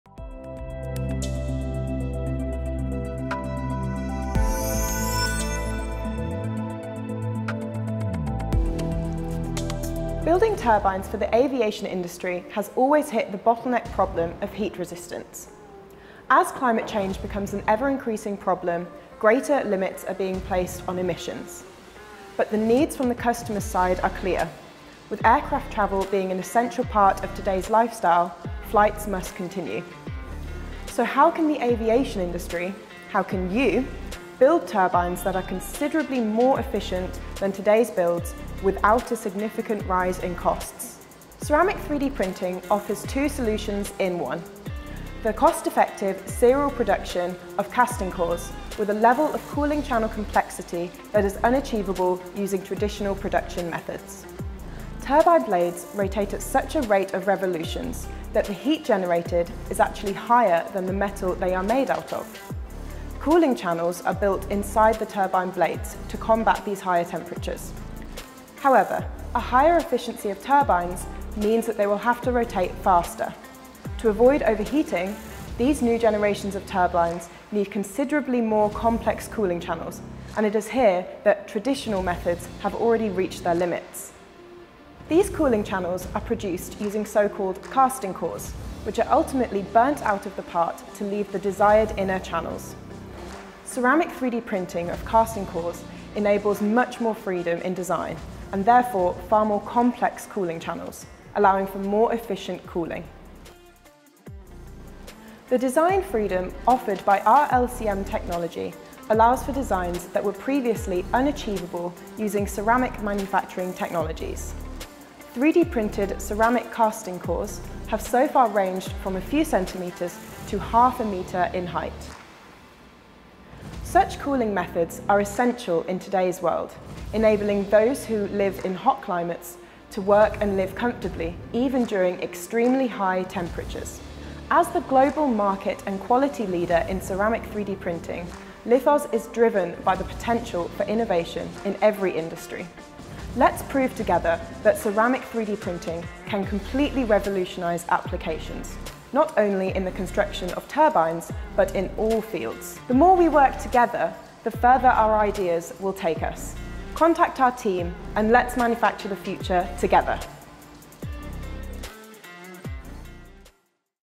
Lithoz Explains #5: Ceramic 3D Printing in Aviation
Ceramic 3D Printing in Aviation: A New Dimension in Heat Resistance

Ceramic materials are being widely used to overcome obstacles such as high temperatures and complex intricacy. But just why are ceramics the answer to such thermal challenges in the aviation industry? Find out in this fifth video of our 'Lithoz Explains' series, all about ceramic 3D printing!